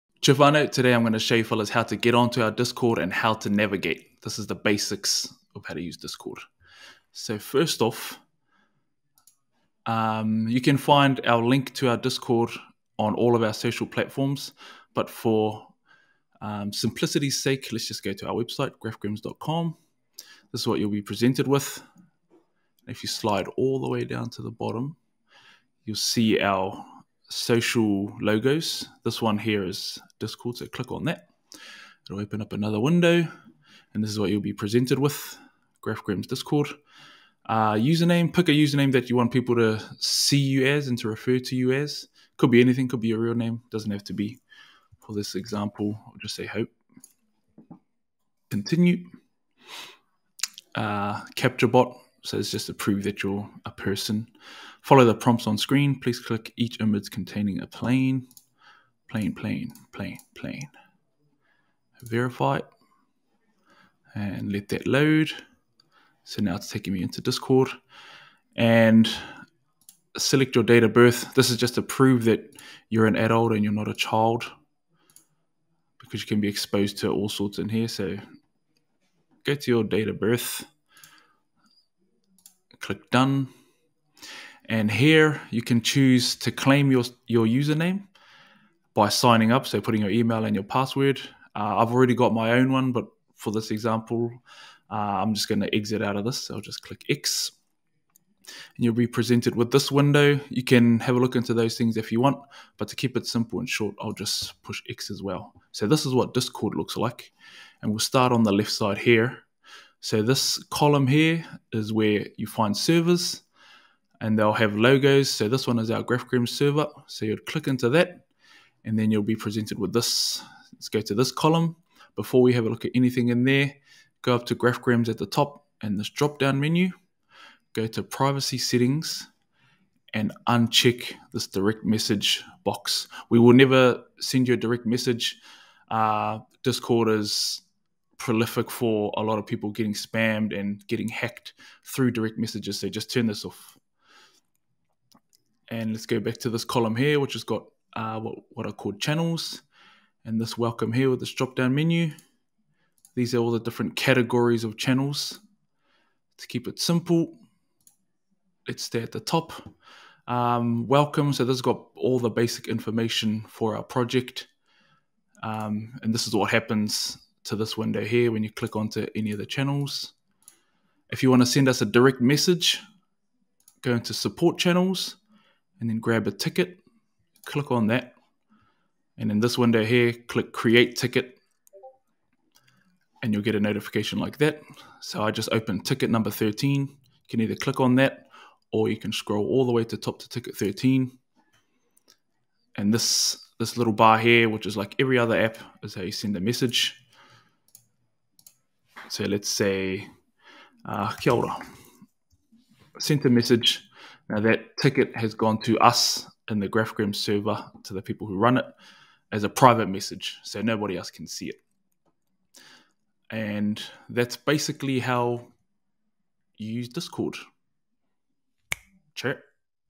Discord basics
If you're brand new to our community and are wondering how to use and navigate Discord, this video is for you. This is the basics of Discord.